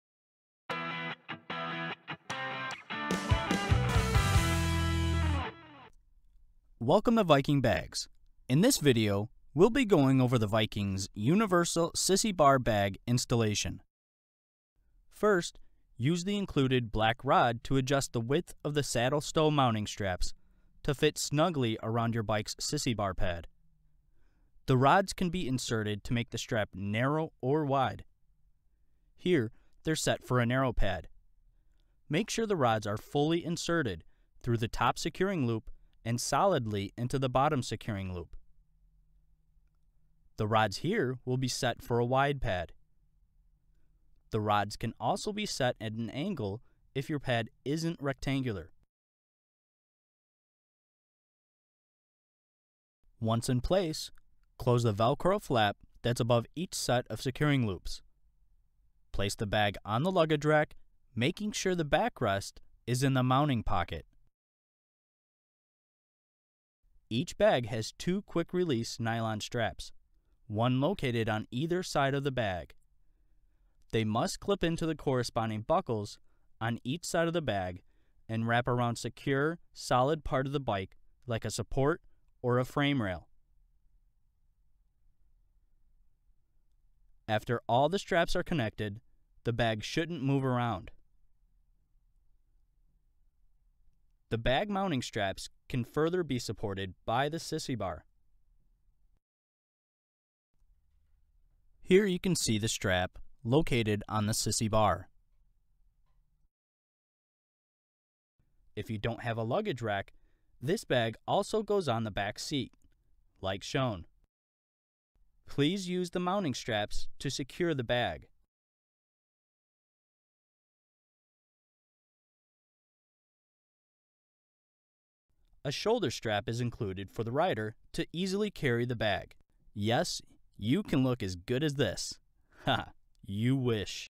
Vikingbags Sissy Bar Bag Installation Instructions- Audio
This video takes you step by step and shows you all the parts needed for you to install our Sissy Bar Bag. This video can be used for most popular Harley-Davison, Honda, Suzuki, Kawasaki, Triumph, Victory and Yamaha models.

To look at our selection of Sissy Bar Bags, please look at our